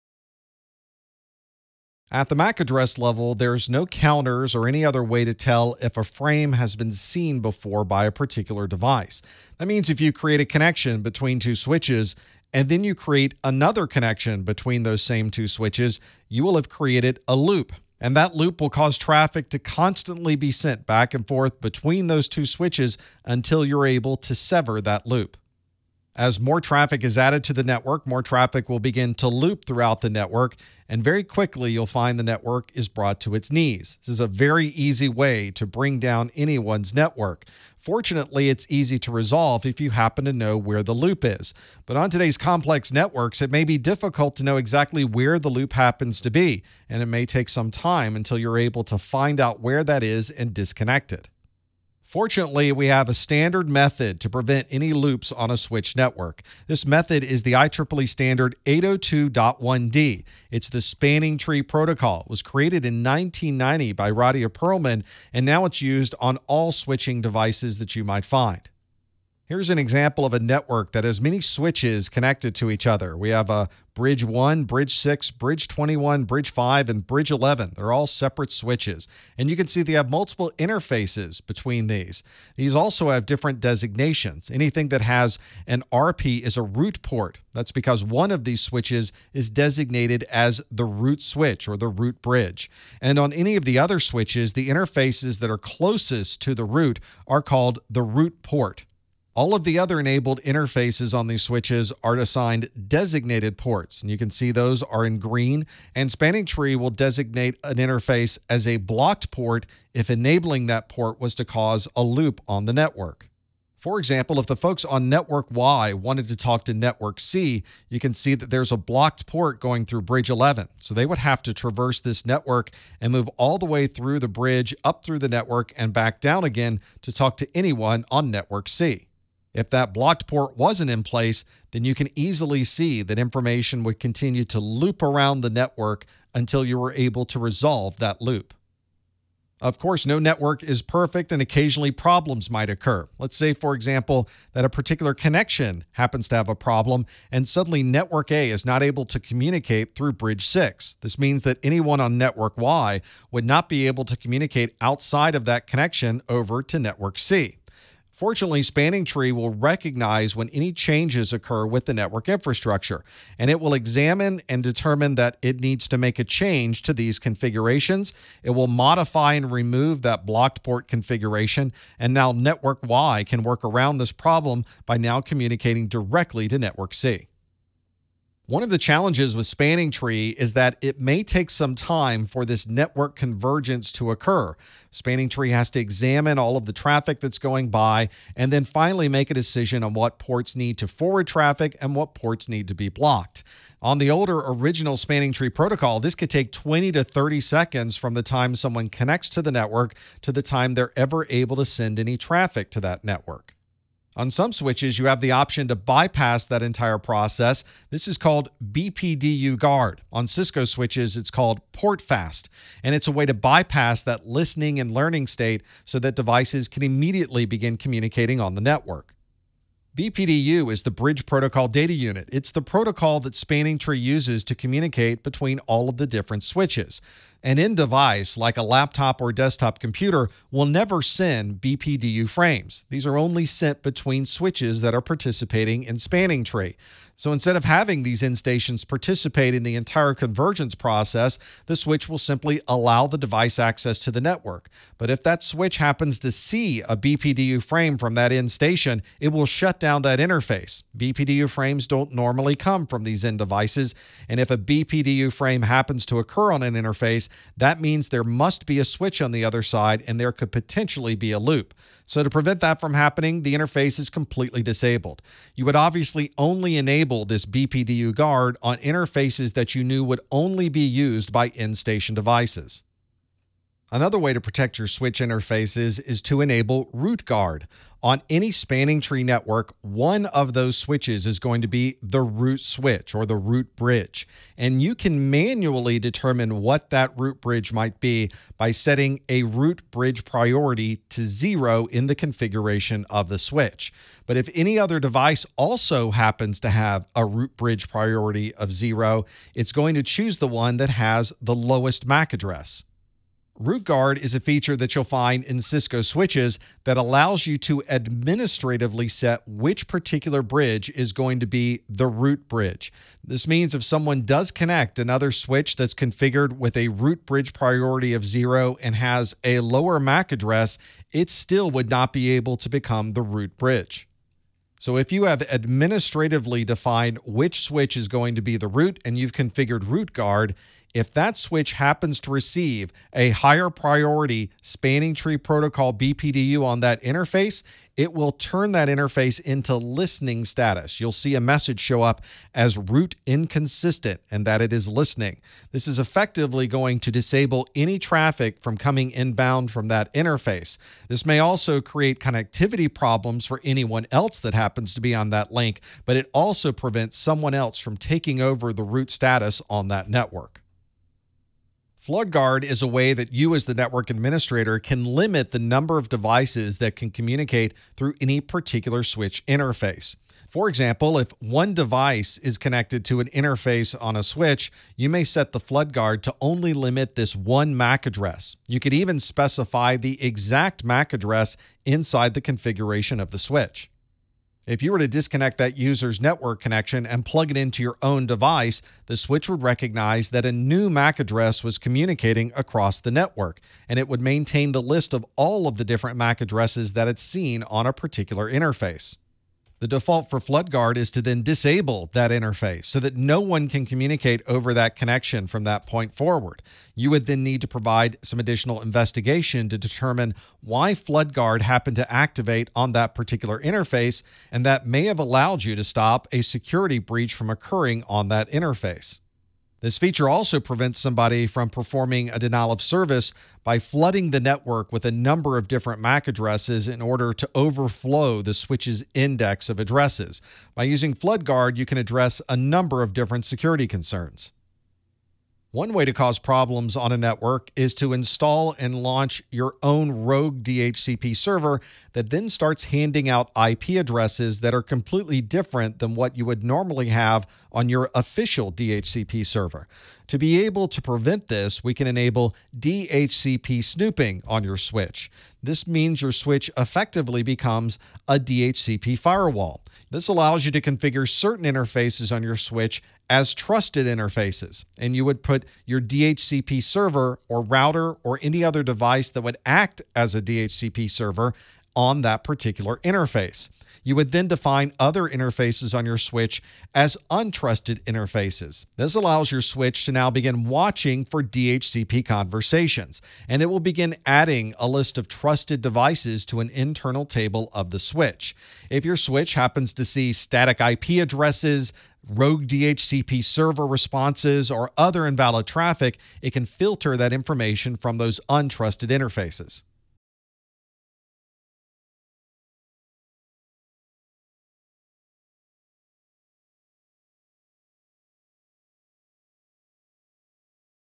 Switch Port Protection - CompTIA Network+ N10-007 - 4.6
If you manage switches, then you’ll need to understand the best way to secure your switched infrastructure. In this video, you’ll learn about Spanning Tree Protocol, BPDU guard, root guard, and more.